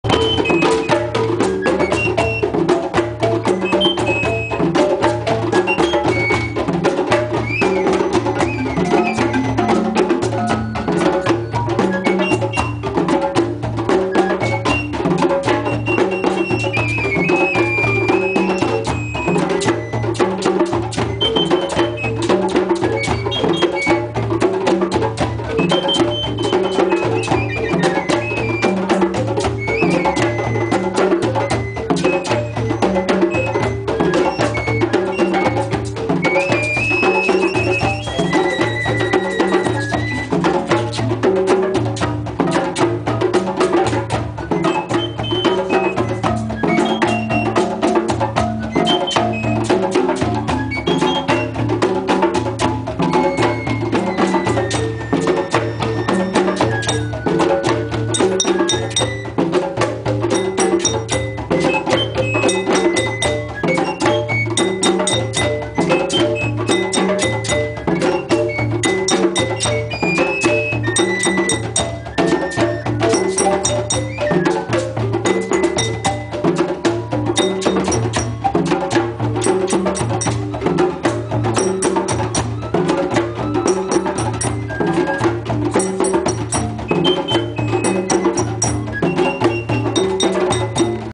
Busking in Middletown
OGADE busking in Middletown, Ohio on May 25, 2007